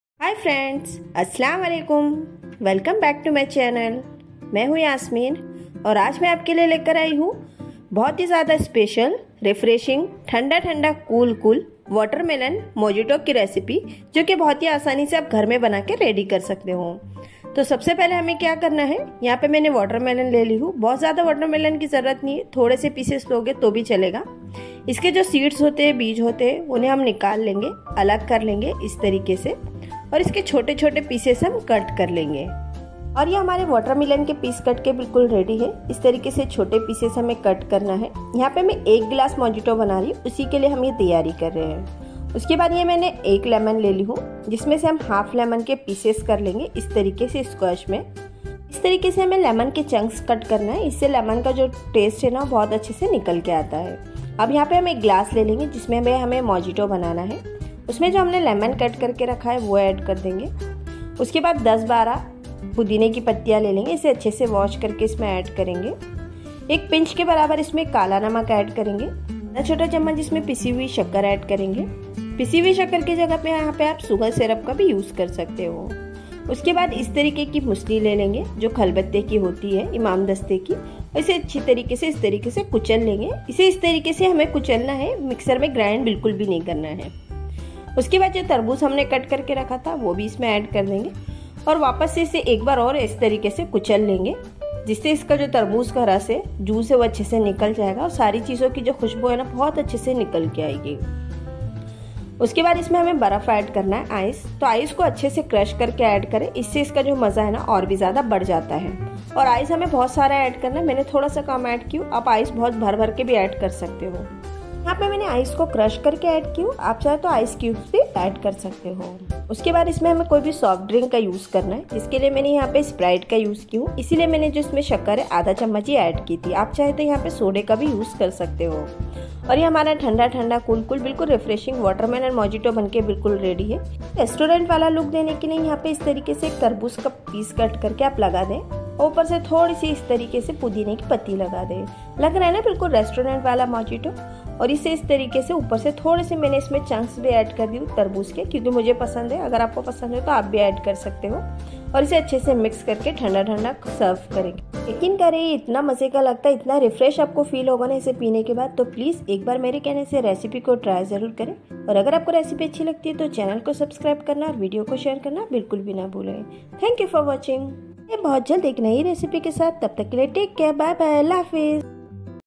तरबूज फ्रैश मोजिटो।watermelon mojito.watermelon refreshing juice/mocktail recipe 5mint drink recipe.
Recipe Dil se with Yasmin
tarbooz juice
watermelon mojito
watermelon mocktail
watermelon refreshing drink